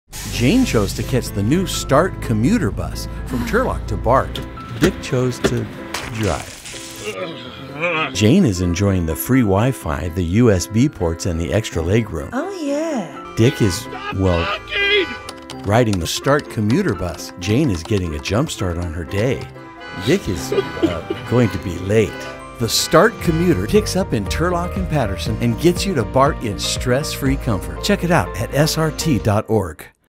StaRT Commuter
Introducing Stanislaus Regional Transit's NEW Commuter service. Now, commuters can ride in air-conditioned, stress-free comfort from Turlock to Patterson to BART. Featuring free Wifi, USB ports, and tray tables. to learn more!